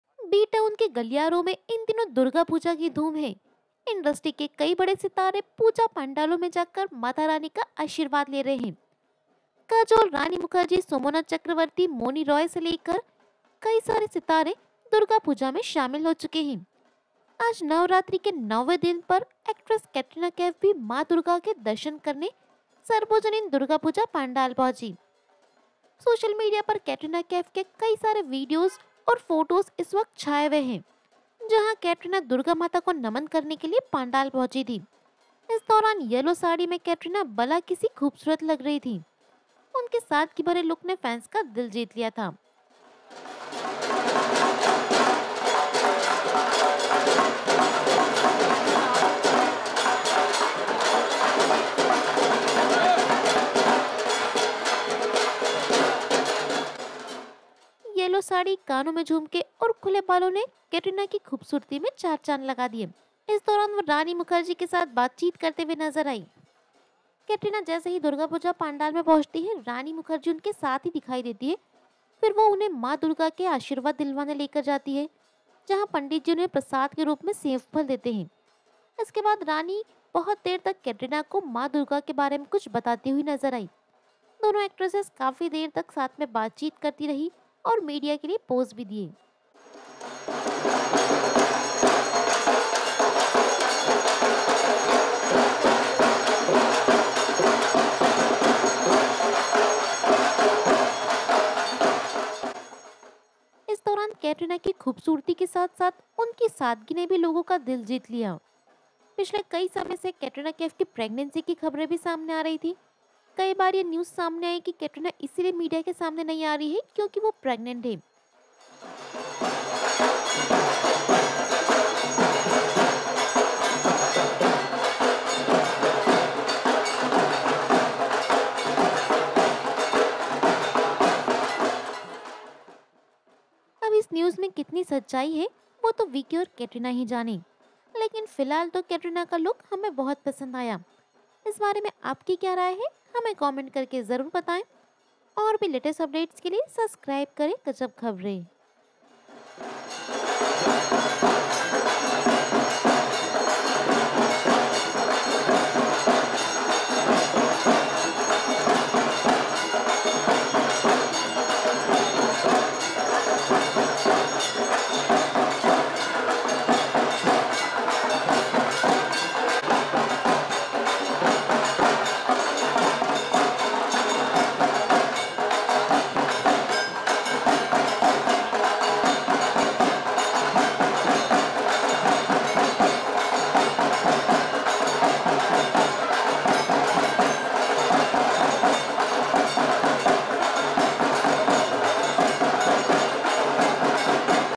Katrina Kaif Looking Gorgeous in Yellow saree poses with Rani Mukerji at Biggest Durga Puja 2023
प्रेग्नेंसी रूमर्स के बीच दुर्गा पूजा में शामिल हुईं कटीरना कैफ, येलो साड़ी में दिखी खूबसूरत
Katrina Kaif seek blessings from Maa Durga with Rani Mukerji at North Bombay Sarbojanin Durga Puja

बेटी देवी संग माँ दुर्गा के दर्शन करने पहुंचे बिपाशा करण, लाल साड़ी में परी दिखी दिखी नन्ही सी देवी